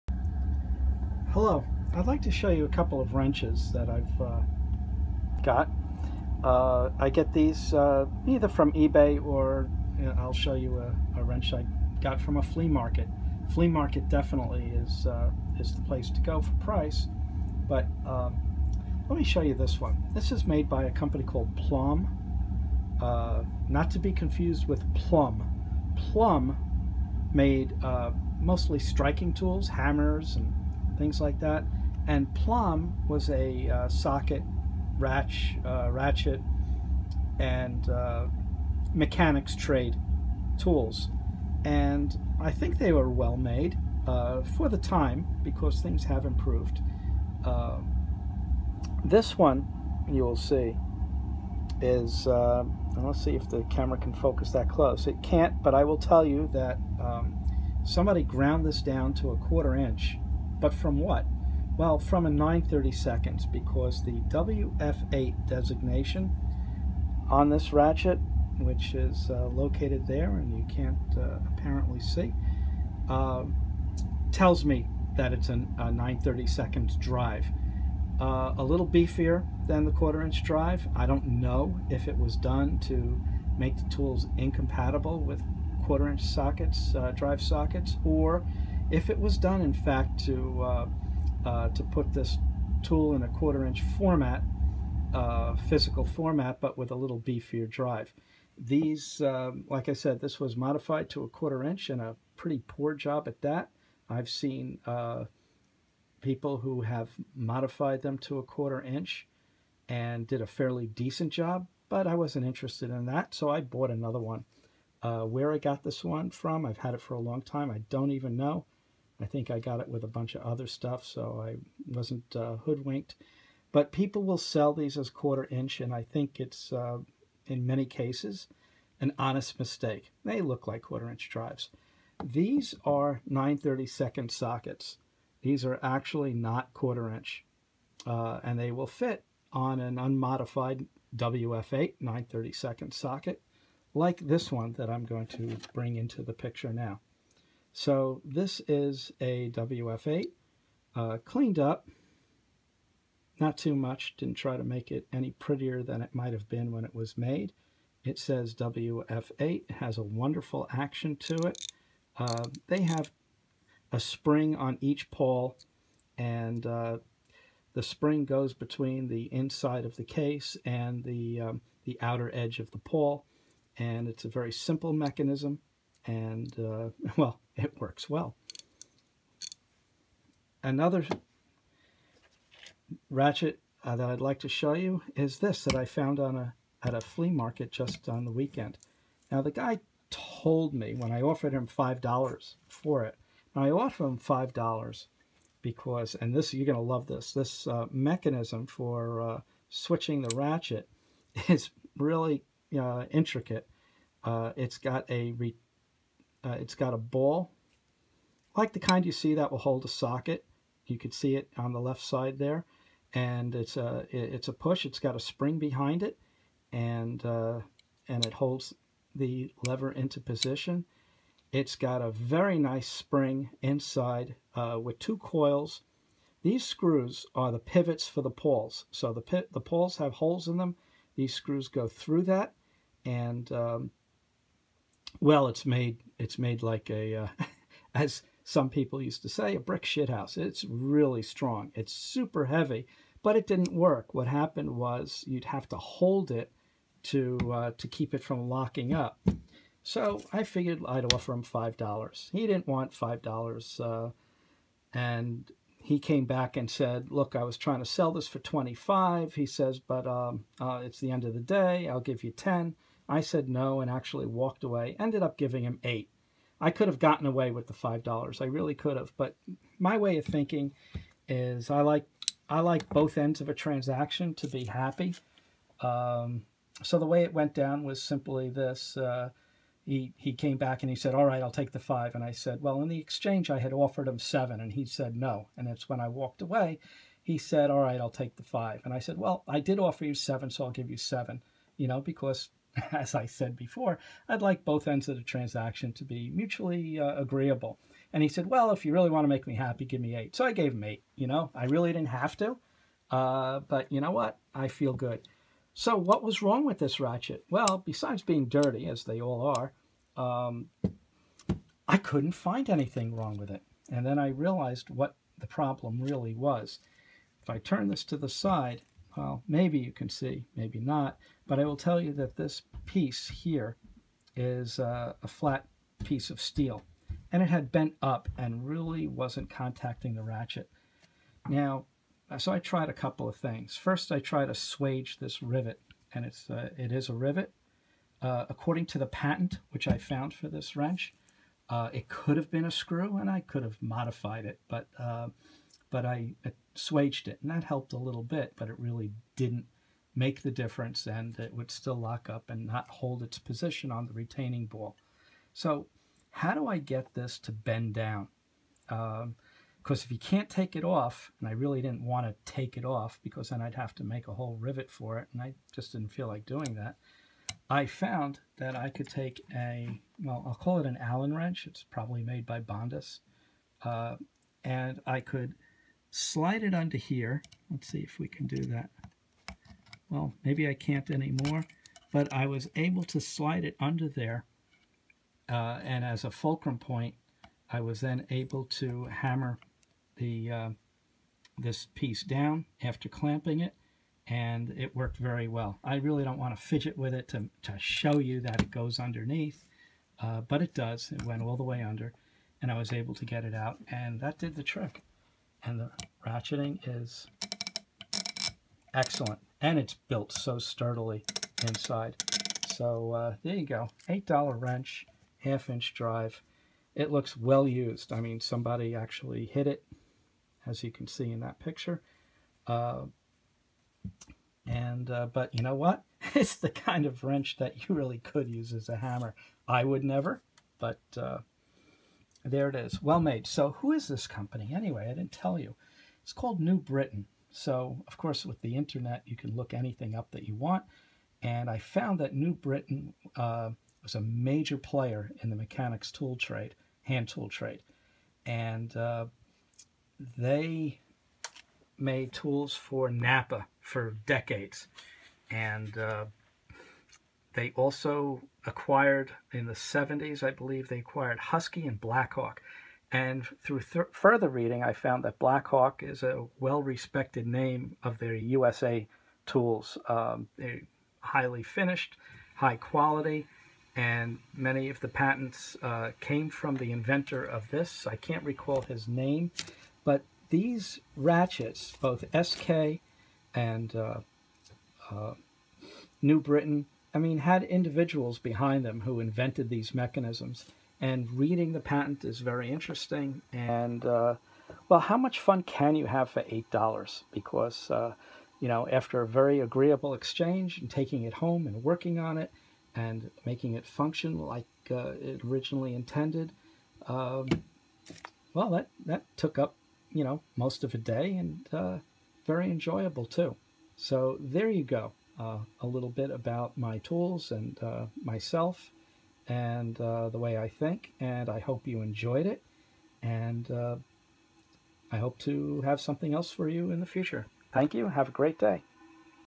Plomb and New Britain ratchet show & tell
A show & tell about these vintage tools which are a little different than the usual stuff you come across. Especially the 1/2 inch drive New Britain. It has an unusual toggle for reversing the ratchet.